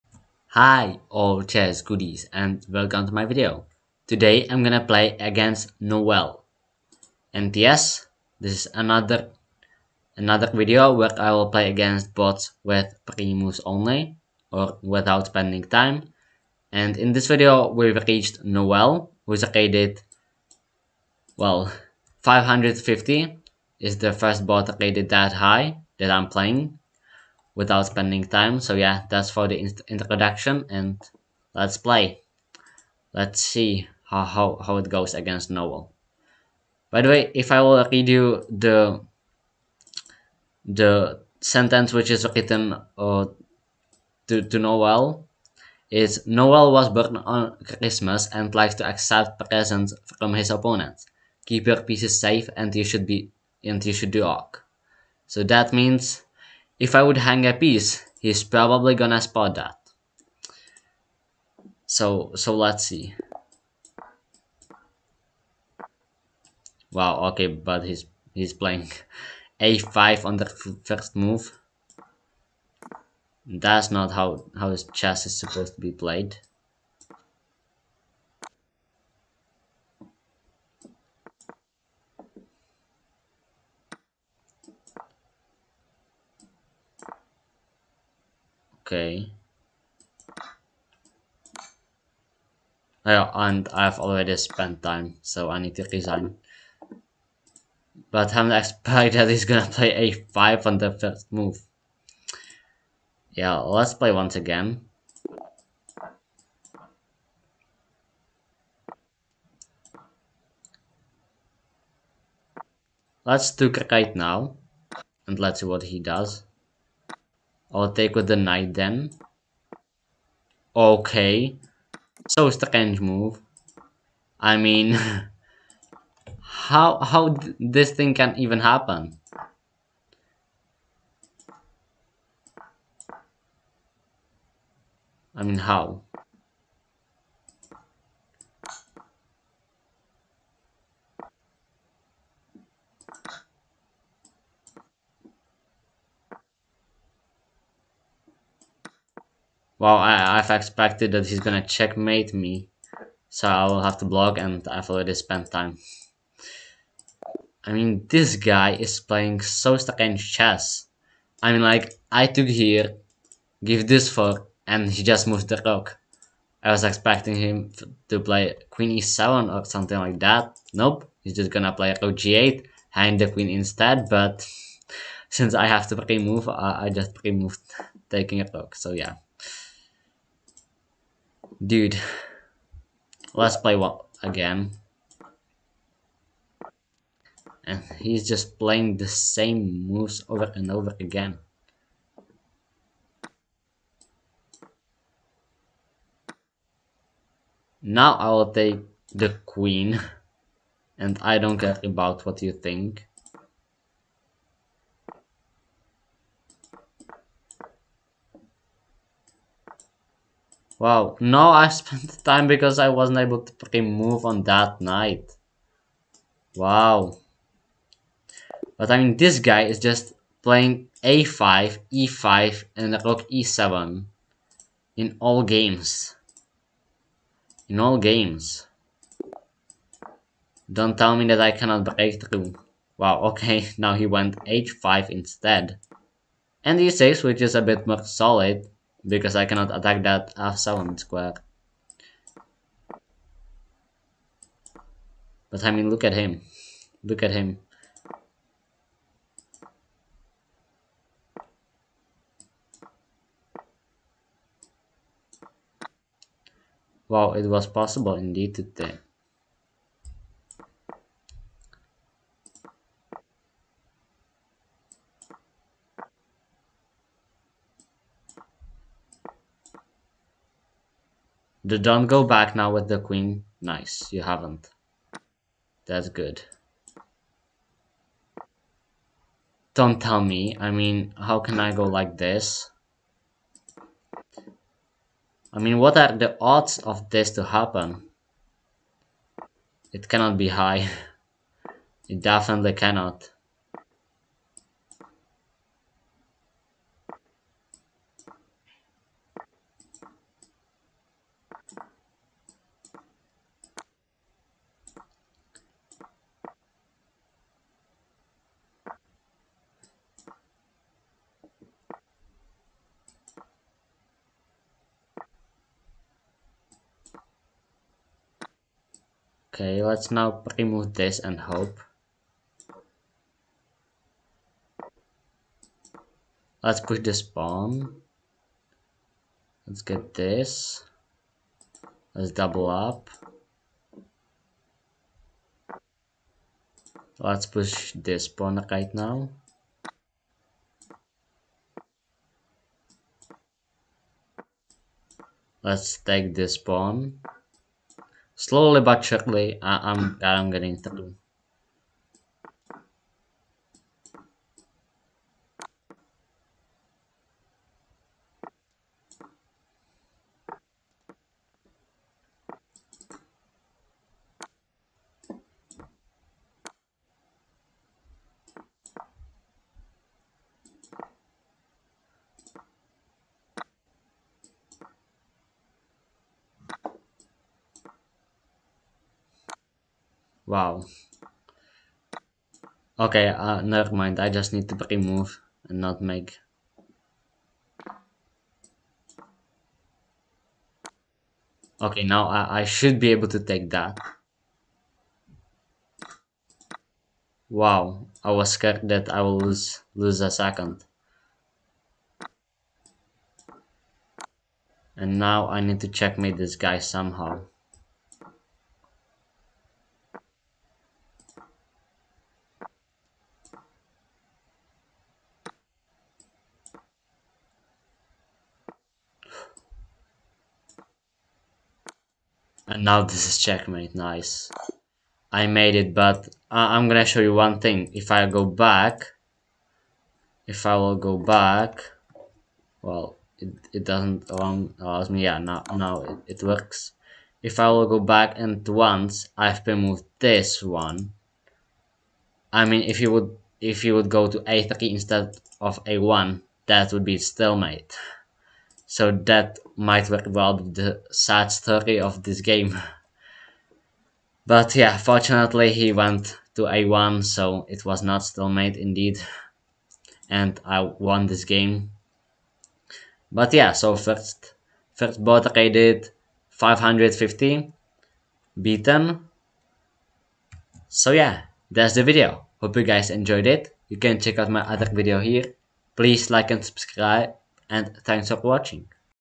It's getting harder......550
Hi, in this video, I'm gonna play against Noel. He's rated 550, so let's see if I can beat him with that little time (0.13 seconds).